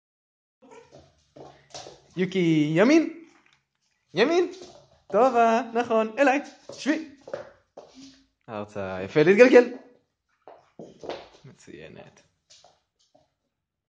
Wait for the END!! Belgian Malinois Puppy Doing Tricks
Wait for the END!! This Belgian Malinois Puppy is 5 1/2 months and is on her way to being a super star!!  Party Tricks all day with her.